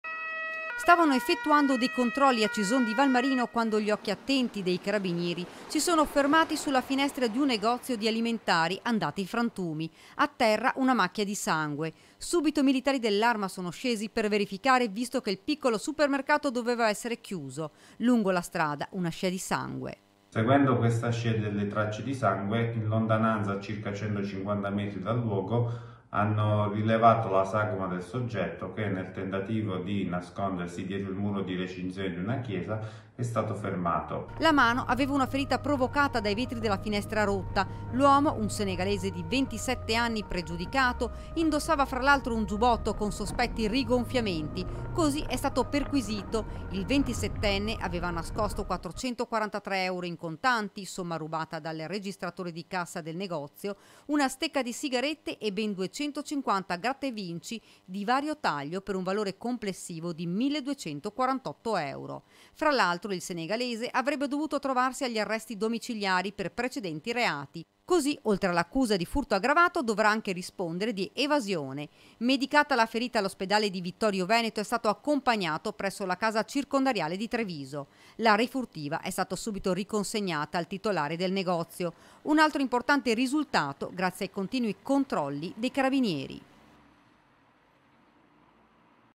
I CARABINIERI SEGUONO LE TRACCE DI SANGUE E ARRESTANO IL LADRO | 13/01/2021
A3 NEWS Treviso 13/01/2021 - CISON DI VALMARINO - Ladro si ferisce rompendo la finestra di un negozio: i carabinieri seguono le tracce di sangue e lo arrestano. E' accaduto a Cison di Valmarino || Stavano effettuando dei controlli, a Cison di Valmarino quando gli occhi attenti dei carabinieri si sono fermati sulla finestra di un negozio di alimentari andata in frantumi. A terra un macchia di sangue. Subito i miliari dell'arma sono scesi per verificare visto che il piccolo supermercato doveva essere chiuso. Lungo la strada una scia di sangue.'' Seguendo le tracce di sangue abbiamo notato un uomo nascosto dietro il muretto della chiesa, spiega Giuseppe Agresti, comandante della Compagnia dei carabinieri di Vittorio Veneto. Lo abbiamo fermato'' La mano aveva un ferita provocata dai vetri della finestra rotta. L'uomo, un senegalese di 27 anni, pregiudicato indossava fra l'altro un giubbotto con sospetti rigonfiamenti. Così è stato perquisito. Il 27 enne aveva nascosto 443 euro in contanti, somma rubata dal registratore di cassa del negozio, una stecca di sigarette e ben 250 gratta e vinci di vario taglio per un valore complessivo di 1248 euro. Fra l'altro, il senegalese avrebbe dovuto trovarsi agli arresti domiciliari per precedenti reati. Così oltre all'accusa di furto aggravato dovrà anche rispondere di evasione. Medicata la ferita all'ospedale di Vittorio Veneto è stato accompagnato presso la casa circondariale di Treviso . La refurtiva è stata subito riconsegnata al titolare del negozio. Un altro importante risultato grazie ai continui controlli dei carabinieri''Abbiamo registrato dice Il comandante Agresti, che i dati relativi ai furti sono drasticamente calati rispetto allo stesso periodo degli anni scorsi''. - Intervistati GIUSEPPE AGRESTI (Comandante Compagnia Carabinieri VIttorio Veneto) (Servizio di Daniela Sitzia) - Segui Antenna Tre anche sul digitale terrestre! Visita il sito